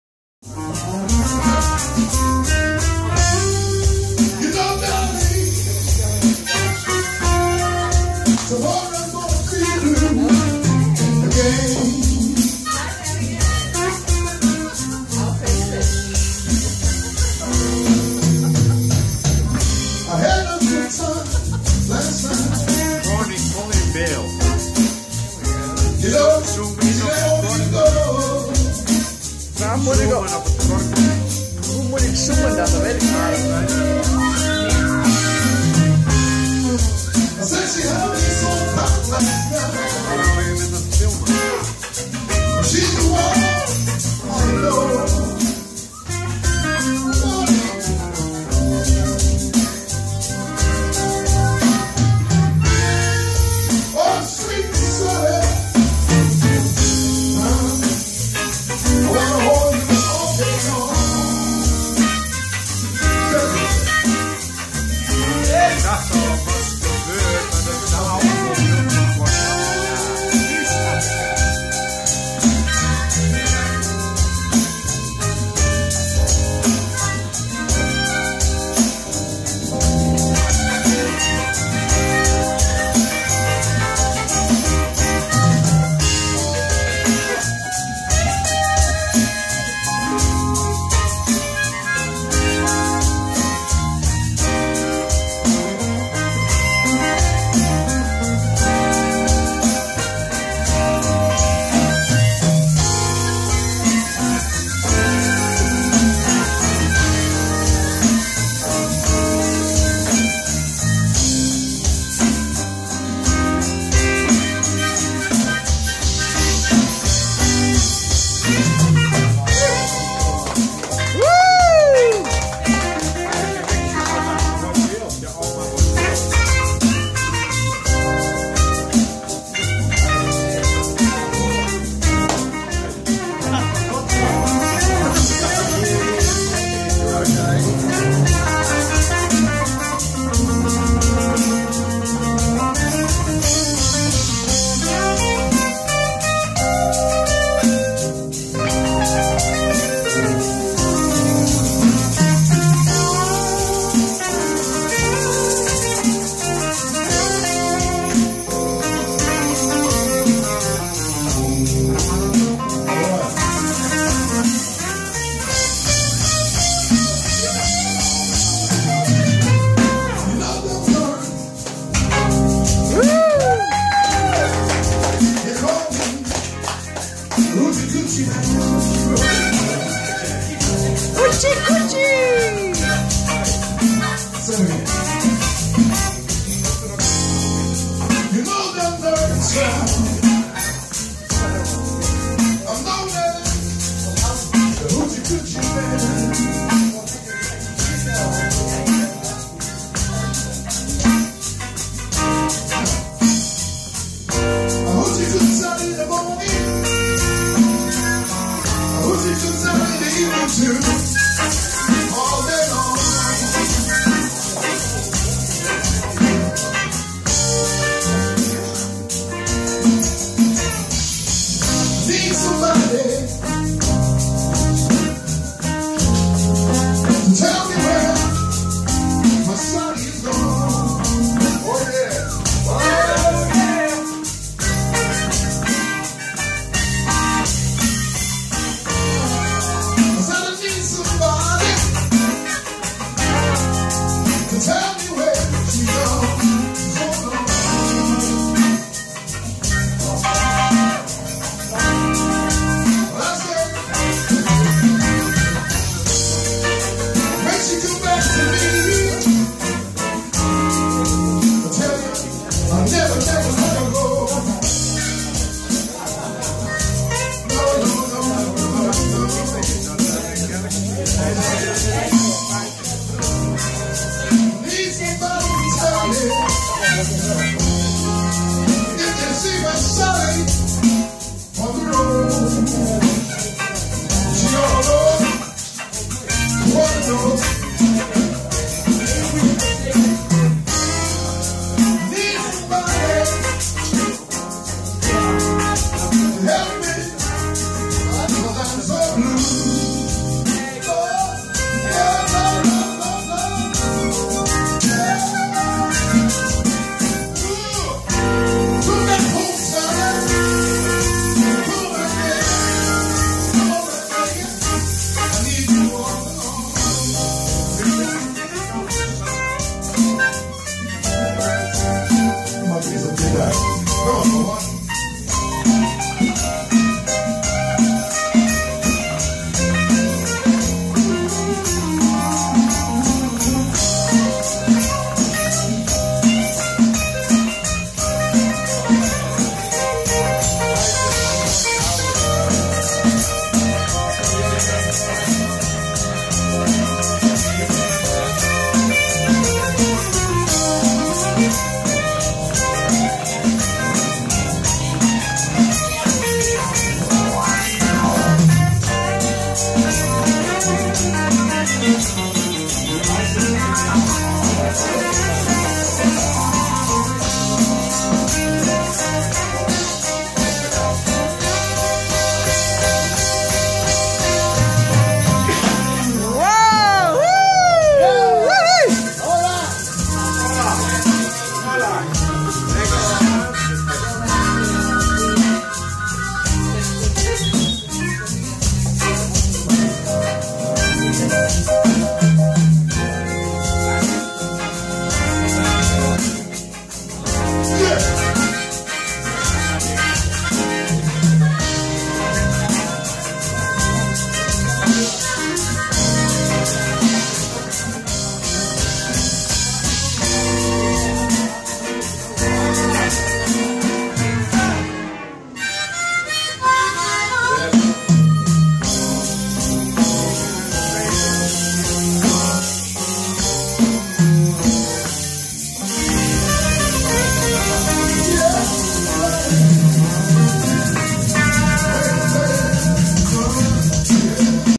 Open podium New Orleans Jazz Cafe. Playing now a blues for a change.
Video van Cornjan Mulder